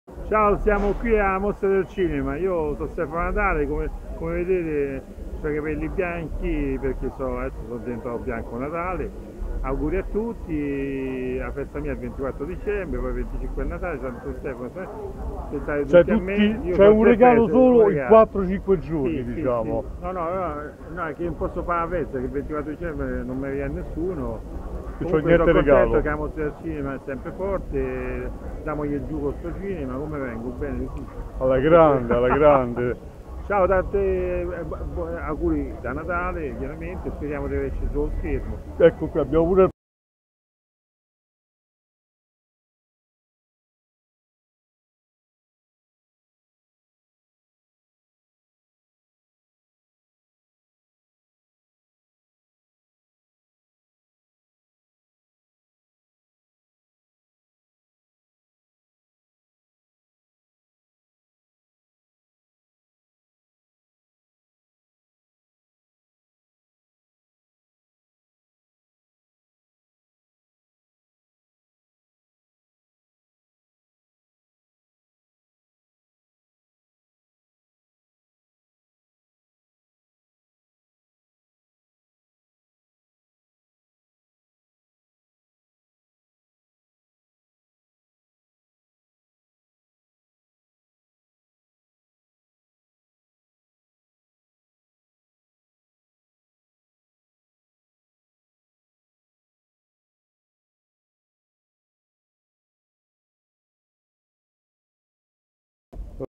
Stefano Natale saluta col Salutatore e il Prodefernando con la sua disavventura con Carlo Verdone
Stefano Natale saluta dal Red Carpet i fan col Salutatore e il Prodefernando con la sua disavventura con Carlo Verdone
➜ FACEBOOK IL SALUTATORE:
➜ PAGINA